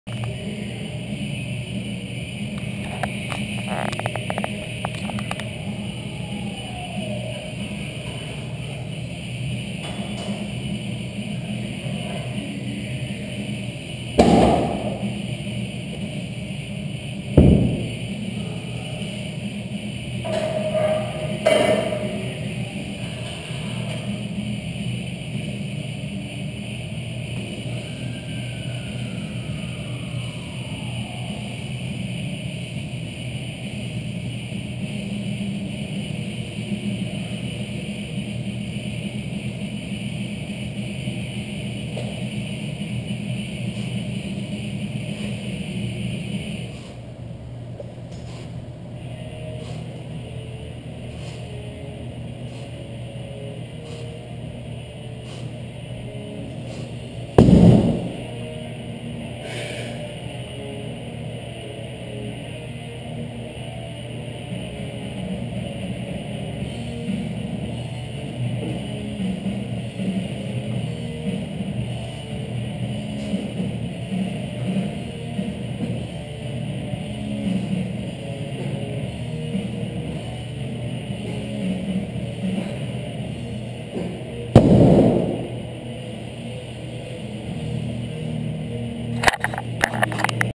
Casey enjoying his 5x10 DB row sets with the 145# DB
Errgg..